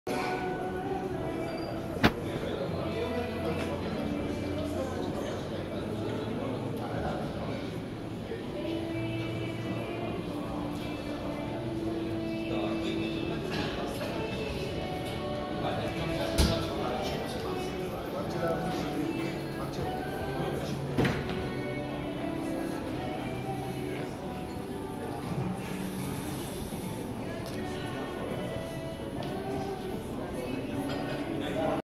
Imagini rare cu Marcel Ciolacu si Kelemen Hunor vorbind separat la Asociatia Municipiilor
Video: Cristian Otopeanu. Lucrările Adunarii Generale a Asociatiei Municipiilor din Romania, la Grand Hotel Bucharest, fostul Hotel Intercontinental. Liderii coalitiei, Marcel Ciolacu si Kelemen Hunor, impreuna.